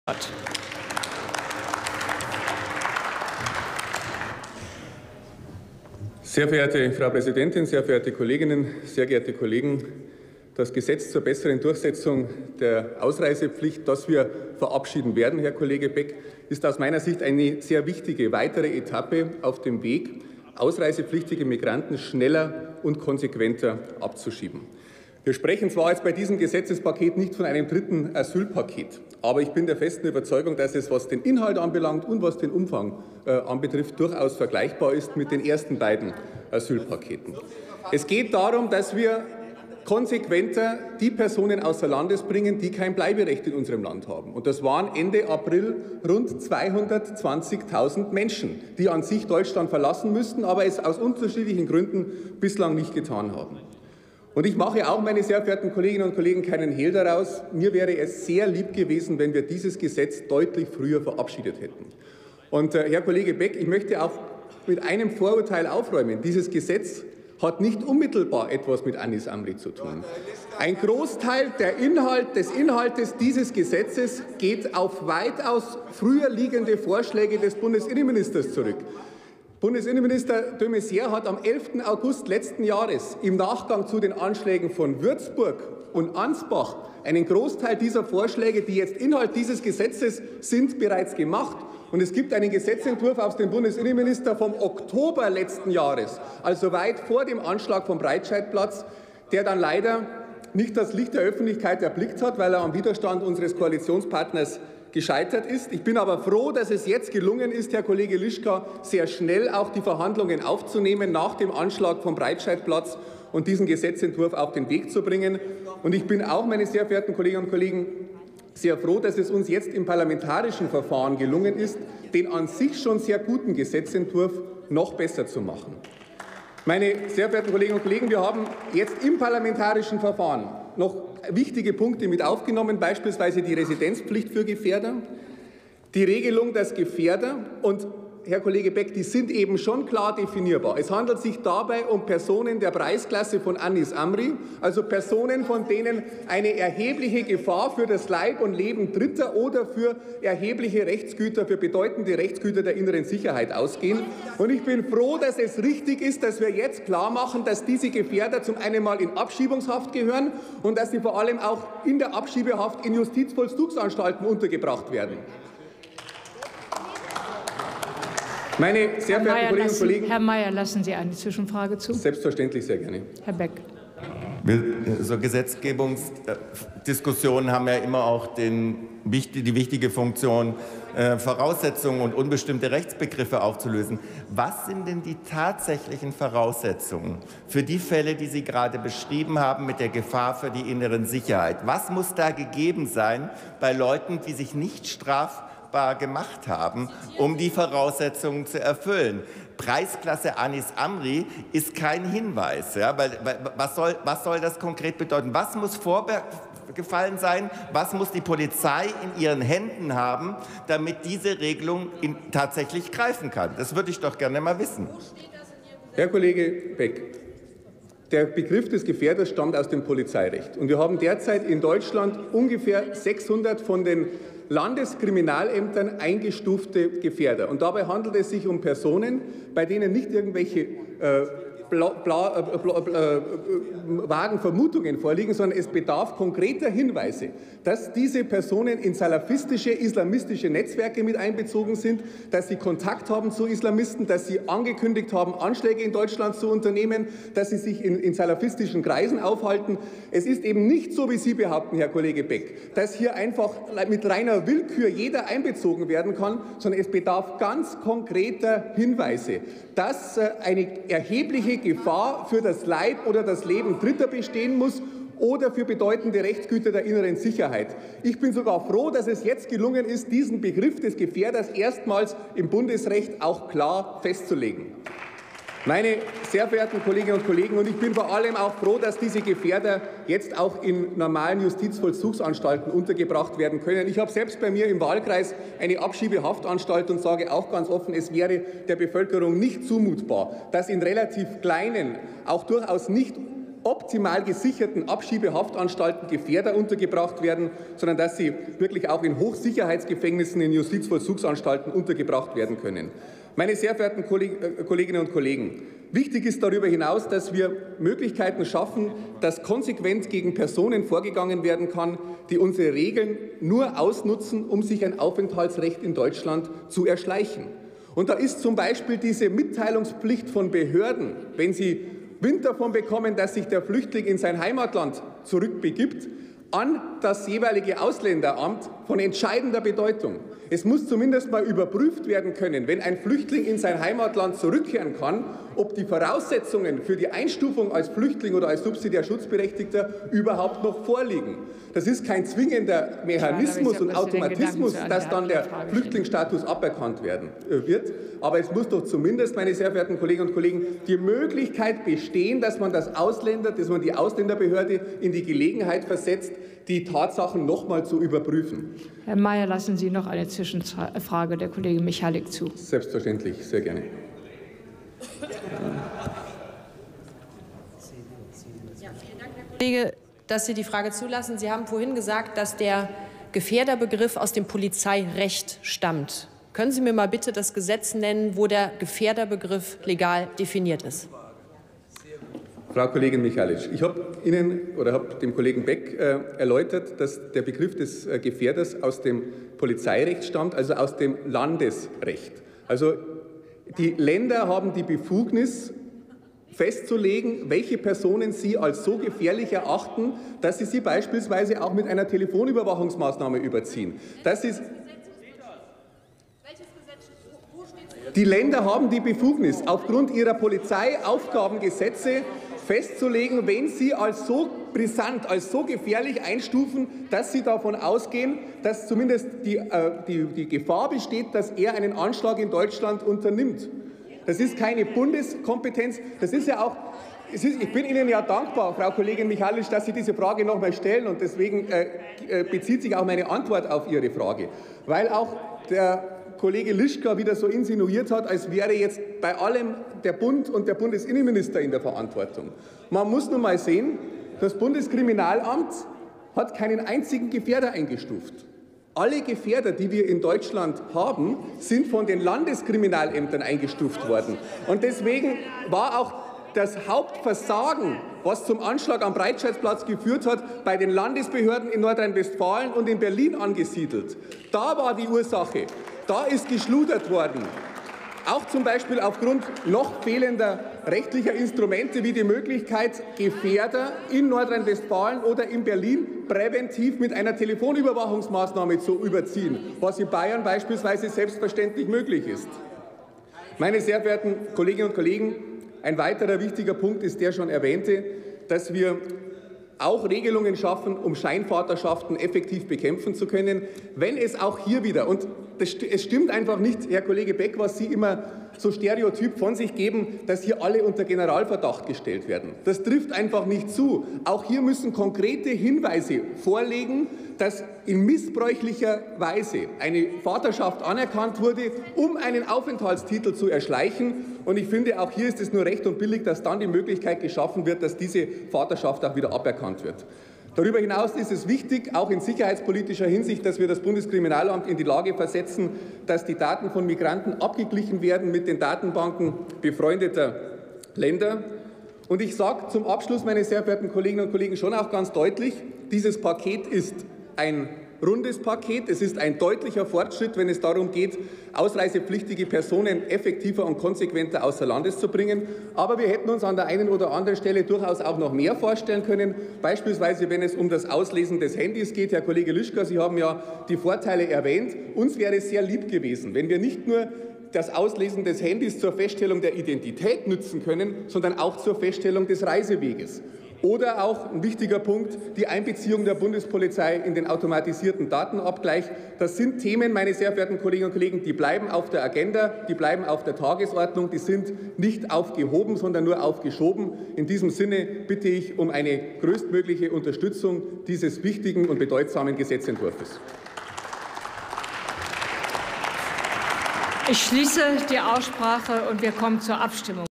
Stephan Mayer: Durchsetzung der Ausreisepflicht [Bundestag 18.05.2017]
eingebrachten Entwurfs eines Gesetzes zur besseren Durchsetzung der Ausreisepflicht
Drucksachen 18/11546, 18/11654, 18/11822 Nr. 9
Beschlussempfehlung und Bericht des Innenausschusses (4. Ausschuss)
Drucksache 18/12415
Bemerkungen
21:44:26 Zwischenfrage: Beck, Volker (B90/Grüne)
21:47:36 Zwischenfrage: Michalk, Maria (CDU/CSU)

18. Deutscher Bundestag
234. Plenarsitzung
Tagesordnungspunkt: Durchsetzung der Ausreisepflicht
Redner: Mayer (Altötting), Stephan, CDU/CSU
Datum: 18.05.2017
Plenarprotokoll:
UVID:7111165